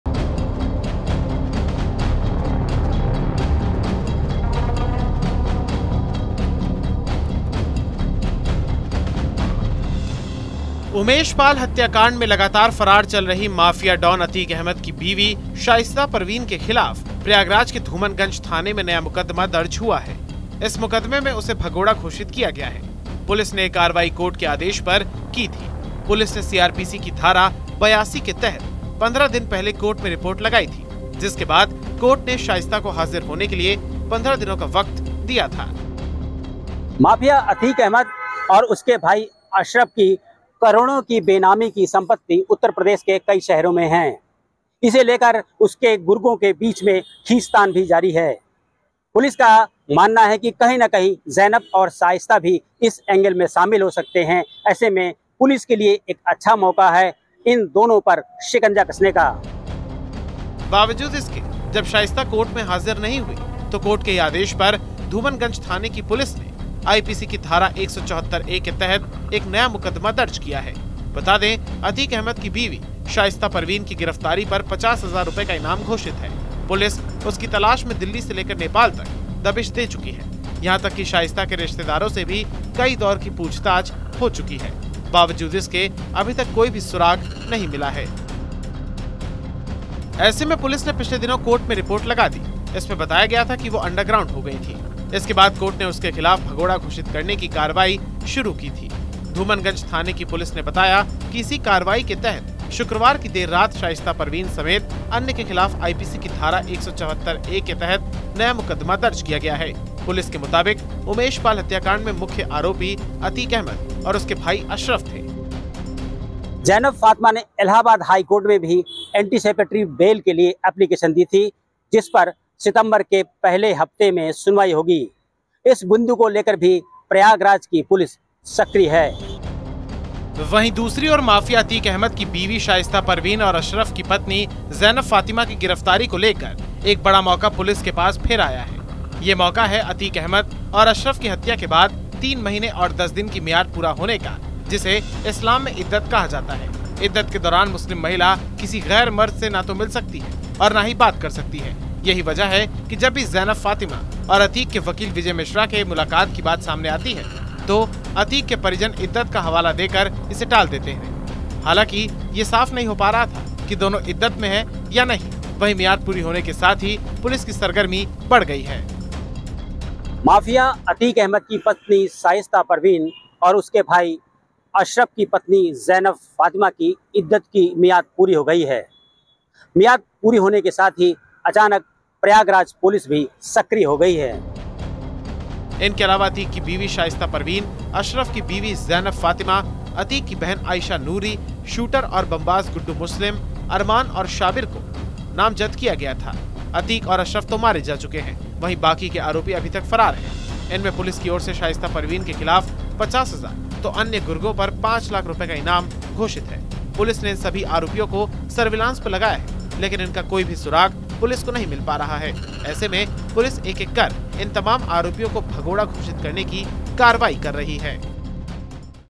Shaista Parveen & Zainab Fatima: शाइस्ता-जैनब को पकड़ने के लिए पुलिस के पास मौका,अब क्या करेगी पुलिस
उमेशपाल हत्याकांड में लगातार फरार चल रही माफिया डॉन अतीक अहमद की बीवी शाइस्ता परवीन के खिलाफ प्रयागराज के धूमनगंज थाने में नया मुकदमा दर्ज हुआ है. इस मुकदमे में उसे भगोड़ा घोषित किया गया है. पुलिस ने यह कार्रवाई कोर्ट के आदेश पर की थी. पुलिस ने सीआरपीसी की धारा 82 के तहत 15 दिन पहले कोर्ट में रिपोर्ट लगाई थी. जिसके बाद कोर्ट ने शाइस्ता को हाजिर होने के लिए 15 दिन का समय दिया था.